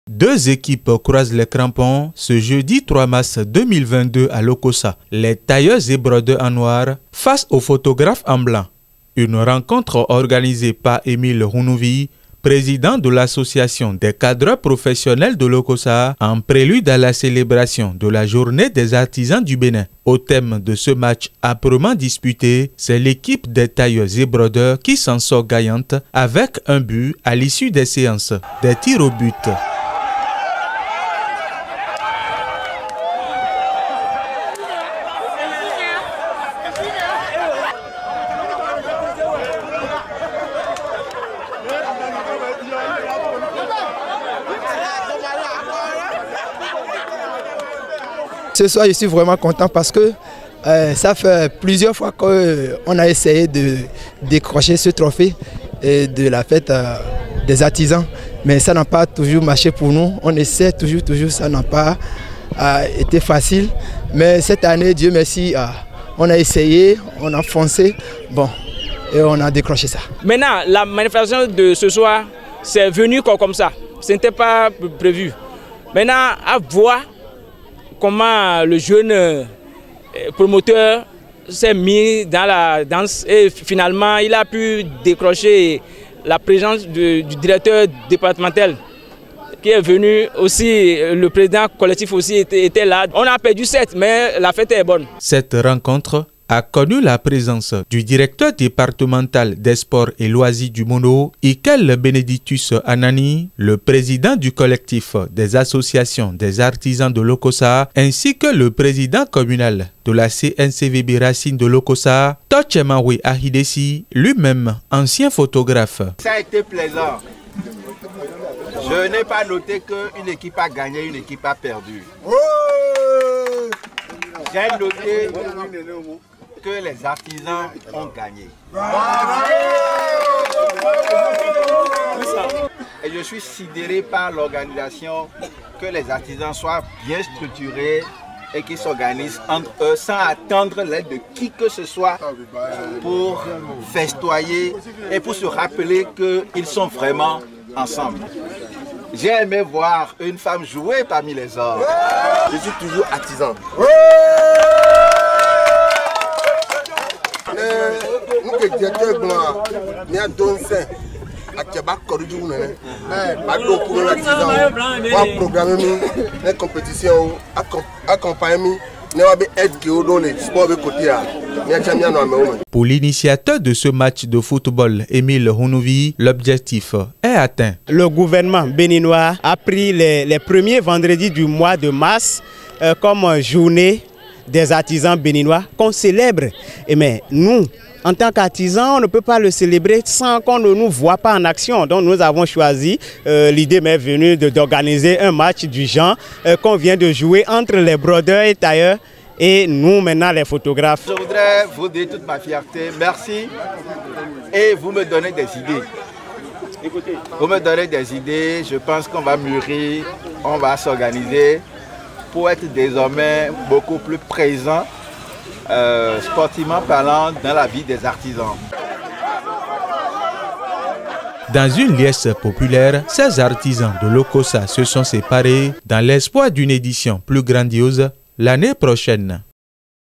JAB 2022/ Football: Les tailleurs et brodeurs remportent le trophée face aux photographes de Lokossa
En prélude à la célébration de la journée des artisans du Bénin, un match de football a opposé à Lokossa les photographes aux brodeurs et tailleurs. Organisée par Émile HOUNNOUVI président de l'ACPL, la rencontre a connue la présence du président du collectif des associations des artisans de Lokossa et du directeur départemental des sports et loisirs du Mono.